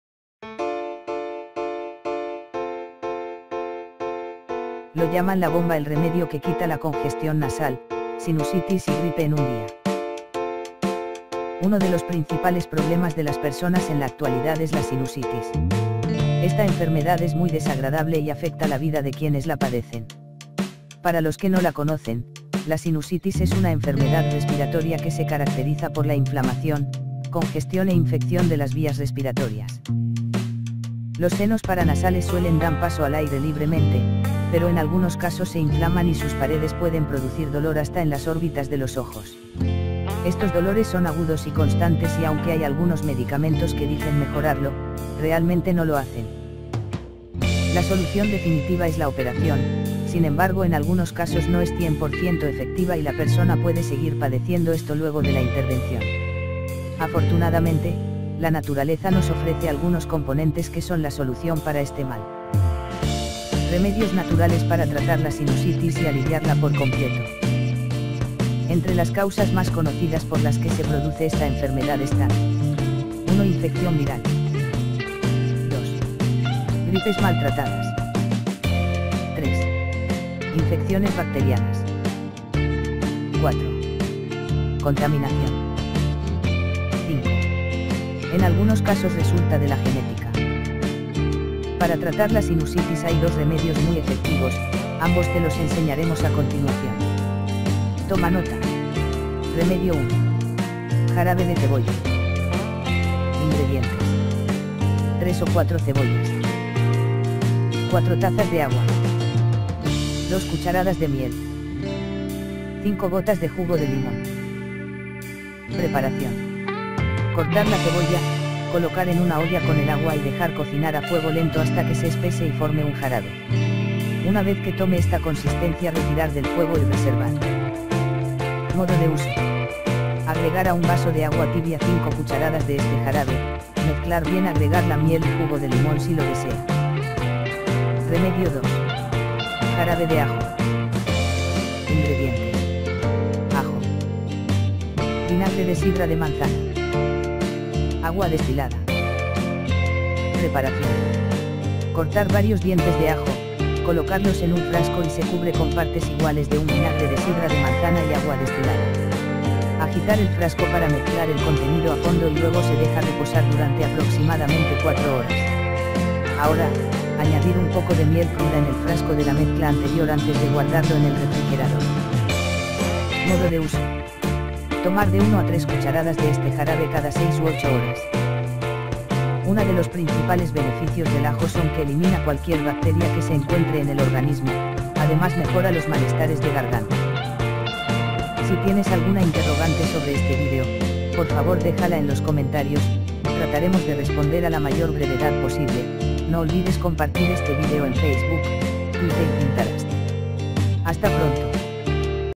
Lo llaman la bomba el remedio que quita la congestión nasal, sinusitis y gripe en un día
Uno de los principales problemas de las personas en la actualidad es la sinusitis. Esta enfermedad es muy desagradable y afecta la vida de quienes la padecen. Para los que no la conocen, la sinusitis es una enfermedad respiratoria que se caracteriza por la inflamación, congestión e infección de las vías respiratorias.

Los senos paranasales suelen dan paso al aire libremente, pero en algunos casos se inflaman y sus paredes pueden producir dolor hasta en las órbitas de los ojos. Estos dolores son agudos y constantes y aunque hay algunos medicamentos que dicen mejorarlo, realmente no lo hacen.

La solución definitiva es la operación, sin embargo en algunos casos no es 200% efectiva y la persona puede seguir padeciendo esto luego de la intervención. Afortunadamente, la naturaleza nos ofrece algunos componentes que son la solución para este mal.

Remedios naturales para tratar la sinusitis y aliviarla por completo

Entre las causas más conocidas por las que se produce esta enfermedad están:
1. Infecciones virales
2. Gripes mal tratadas
3. Infecciones bacterianas
4. Contaminación
5. En algunos casos resulta de la genética
Para tratar la sinusitis hay dos remedios muy efectivos, ambos te los enseñaremos a continuación. Toma nota:

REMEDIO 1. Jarabe de cebolla

Ingredientes:
• 3 o 4 cebollas
• 4 tazas de agua
• 2 cucharadas de miel
• 5 gotas de jugo de limón
Preparación:

Cortar la cebolla, colocar en una olla con el agua y dejar cocinar a fuego lento hasta que se espese y forme un jarabe. Una vez que tome esta consistencia retirar del fuego y reservar.

Modo de uso:

Agregar a un vaso de agua tibia 5 cucharadas de este jarabe, mezclar bien agregar la miel y jugo de limón si lo desea.

REMEDIO 2. Jarabe de ajo

Ingredientes:
• ajo
• vinagre de sidra de manzana
• agua destilada
Preparación:

Cortar varios dientes de ajo, colocarlos en un frasco y se cubre con partes iguales de un vinagre de sidra de manzana  y agua destilada. Agitar el frasco para mezclar el contenido a fondo y luego se deja reposar durante aproximadamente 4 horas. Ahora, añadir un poco de miel cruda en el frasco de la mezcla anterior antes de guardarlo en el refrigerador.

Modo de uso:

Tomar de 1 a 3 cucharadas de este jarabe cada 6 u 8 horas.

Una de los principales beneficios del ajo son que elimina cualquier bacteria que se encuentre en el organismo, además mejora los malestares de garganta.